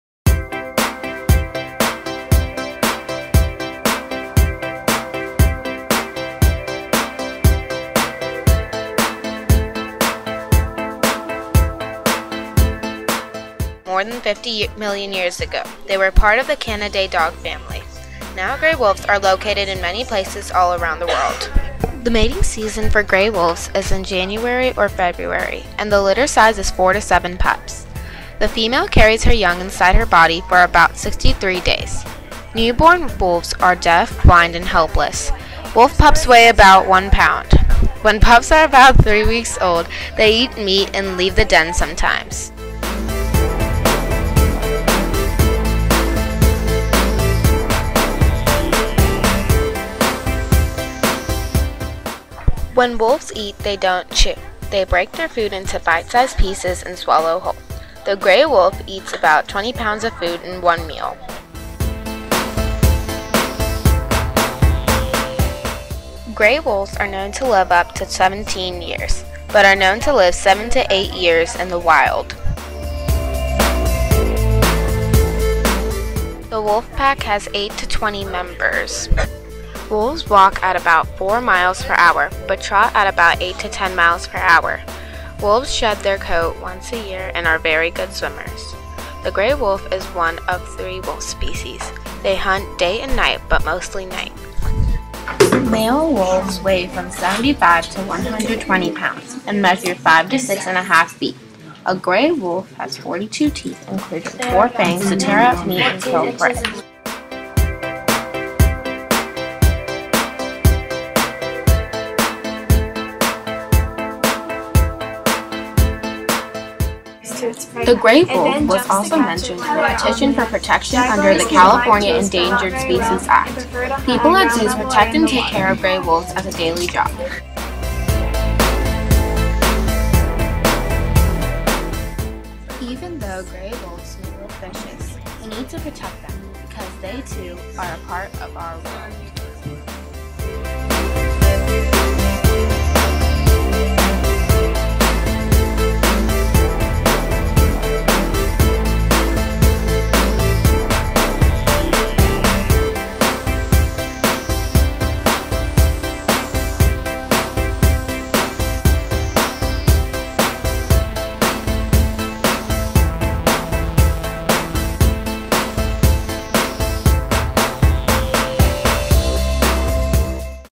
Gray Wolves By:Iman Hirani
This is an IMovie about Gray Wolves. Beware there is a scary picture.